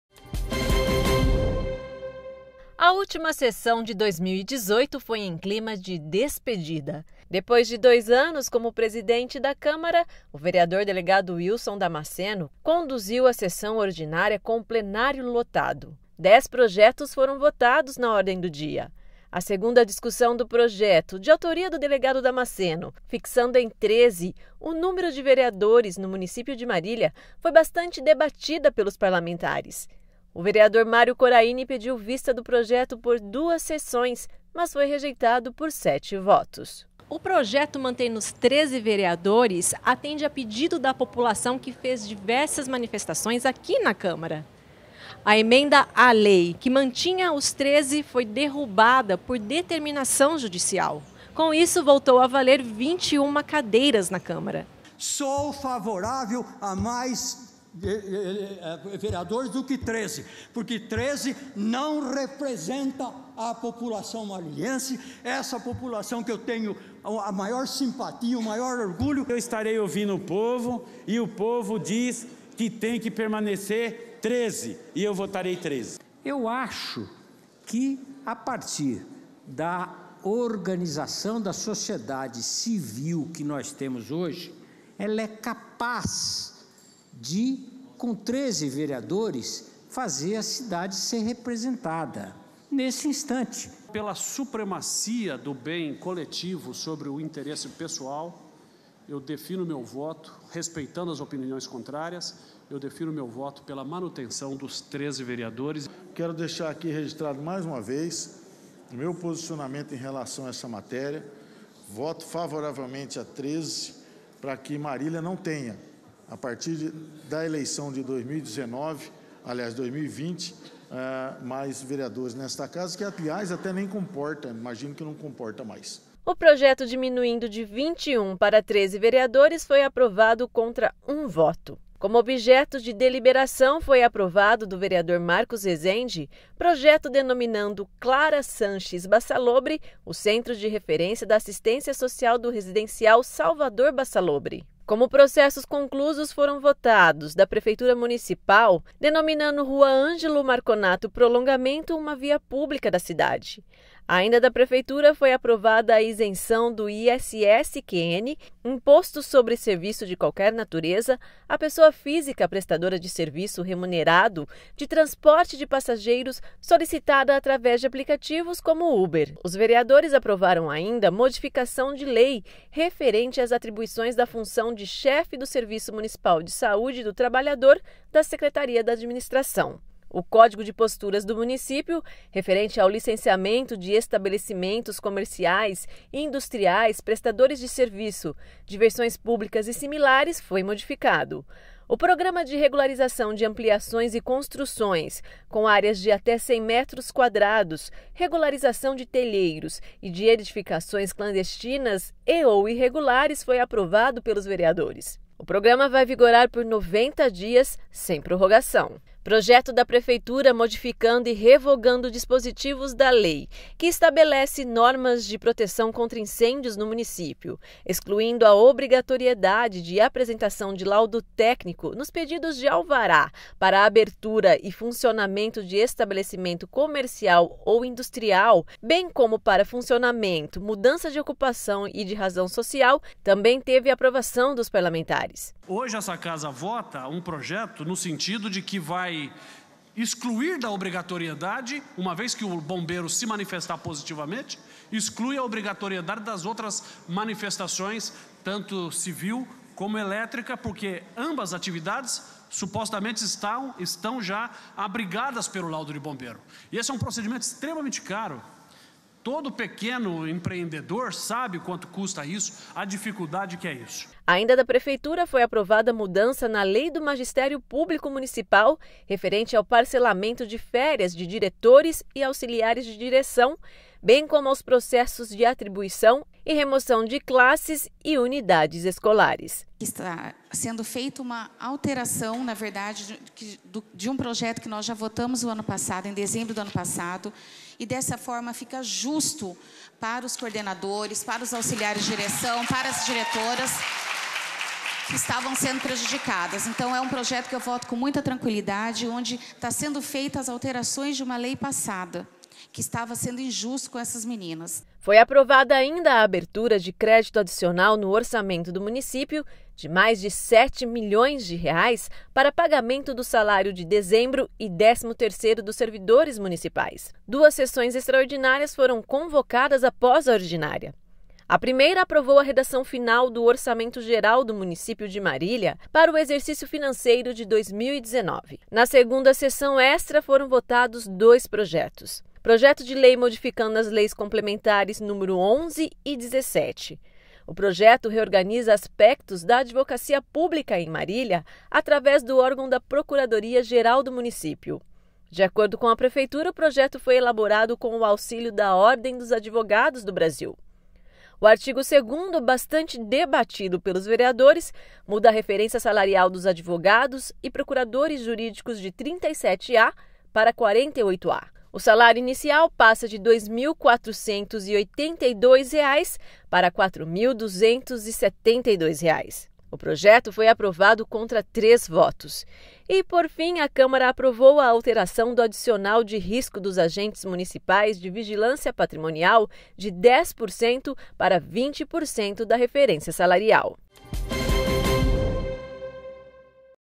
CÂMARA APROVA REDUÇÃO DE 21 PARA 13 VEREADORES
O projeto que reduz de 21 para 13 vereadores, de autoria Delegado Wilson Damasceno, foi aprovado por 12 votos a favor e 1 contra. A última sessão dirigida pelo vereador Damasceno, após dois anos como presidente da Câmara,  teve onze projetos aprovados na ordinária e três em sessão extraordinária.